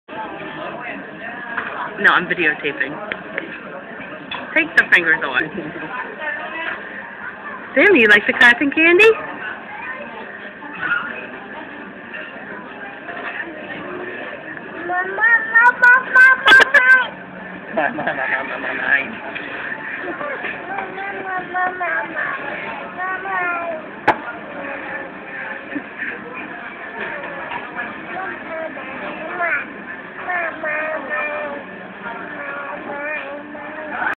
Sammy meets cotton candy
Sammy seems to think its a drum stick instead of something you eat. He never did put it in his mouth.